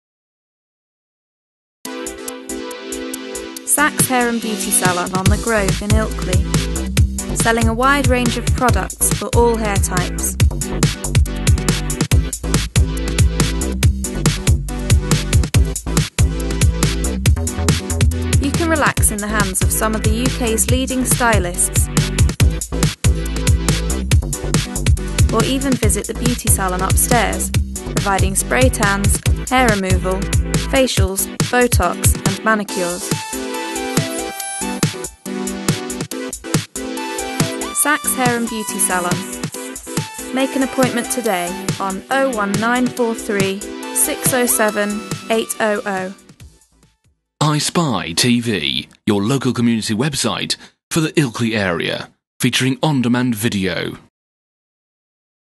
iSPI TV - Saks Hair and Beauty Salon - Ilkley
A very well known chain in hair and beauty, Saks hair and beauty salons are renowned for the excellent service they provide to all men and women. A great team of expert colourists, stylists and beauty therapists make Saks a place you can be confident in the treatments you receive.